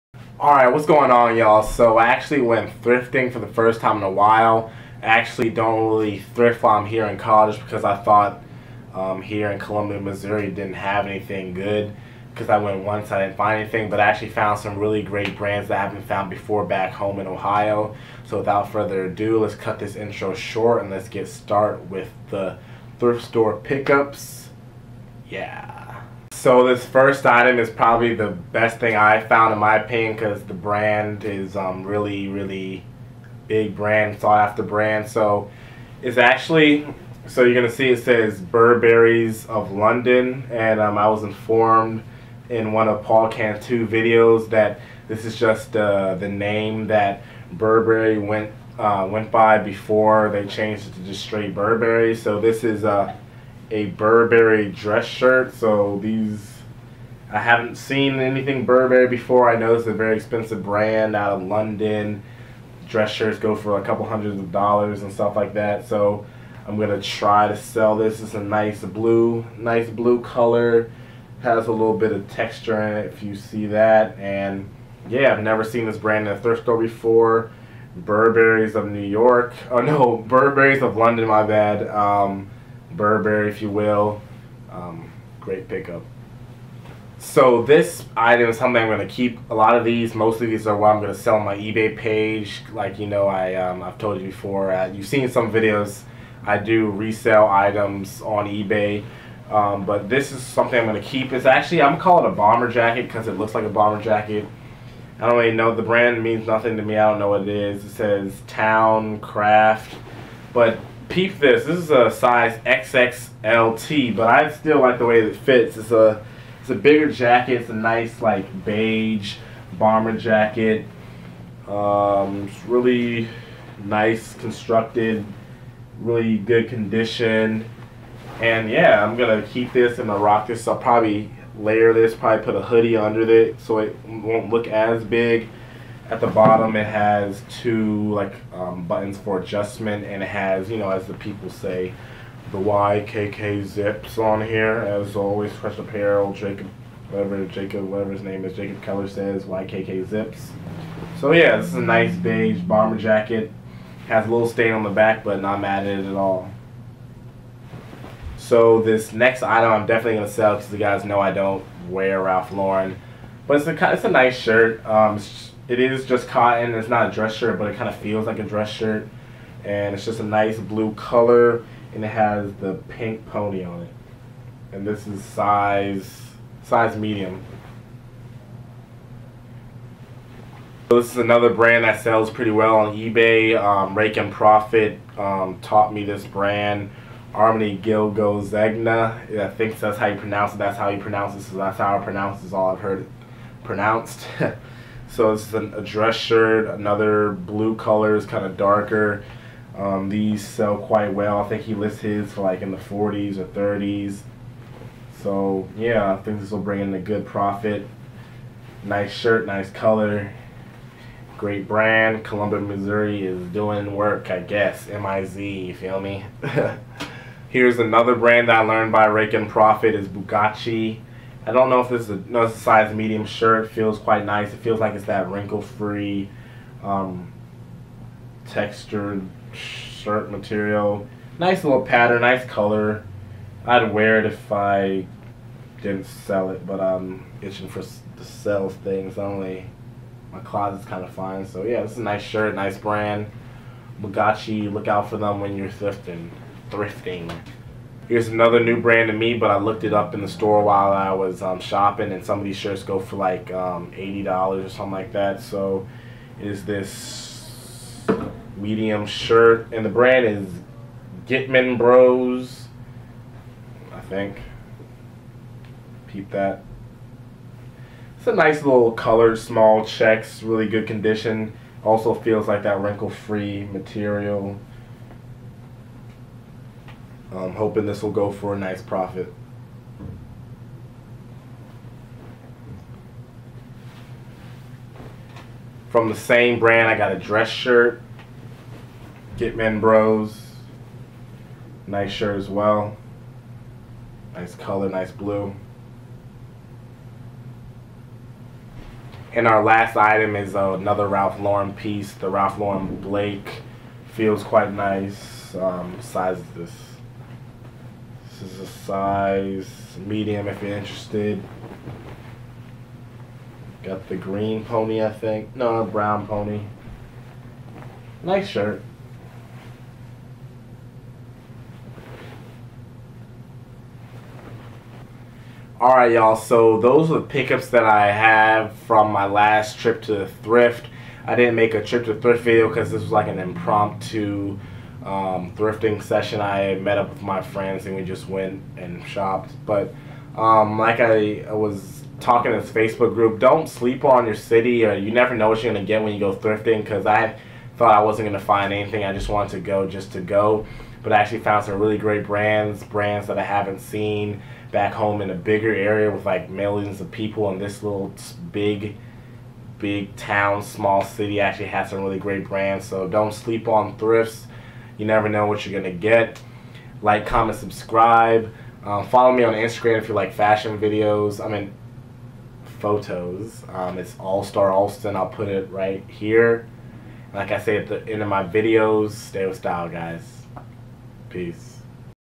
Thrift Store Pickups | Burberry, Bomber Jacket, Ermenegildo Zegna
Found some really great pieces in my college town. Don't sleep on the thrifts, you never know what your going to find.
"STAY WITH STYLE"

Young noble
youngnnoble

trip to the thrift
paul cantu
vintage clothing